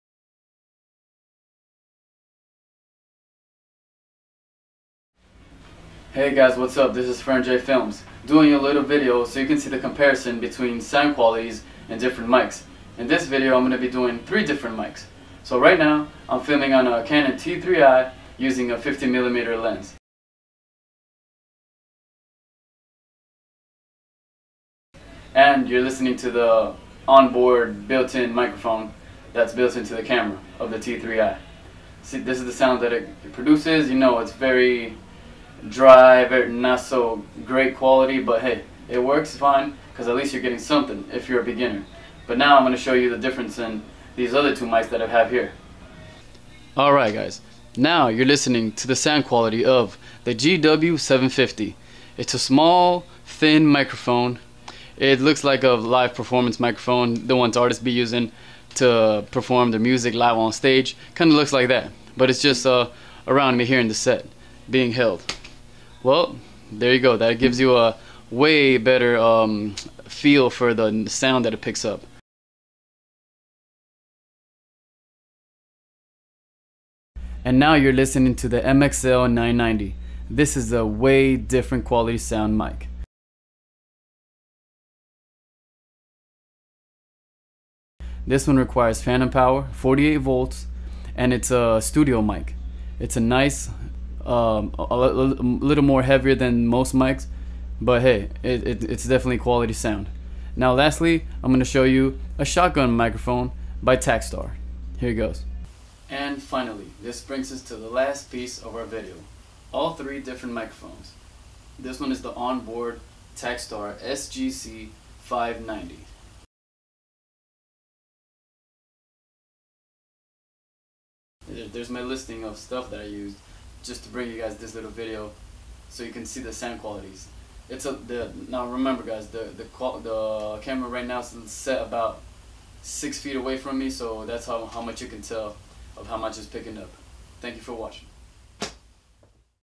4 Microphone Sound Test
Audio Comparison between the following:

-Built In Mic
-Zebra GW-750
-MXL 990
-Takstar SGC-598